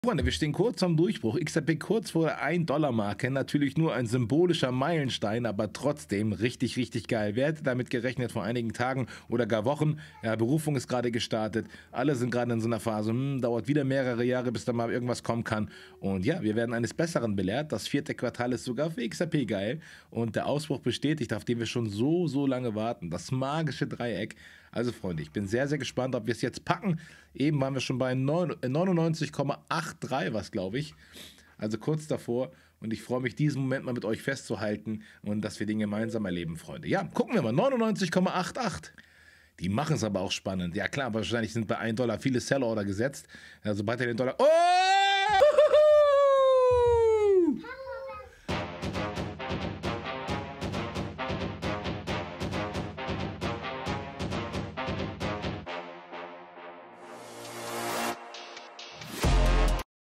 LIVE XRP KNACKT DEN 1$!
WIR HABEN ES UNS VERDIENT NACH ALL DER WARTEZEIT!
DAS IST ERST DER ANFANG!

DANKE FÜR EUREN RÜCKHALT ALL DIE JAHRE!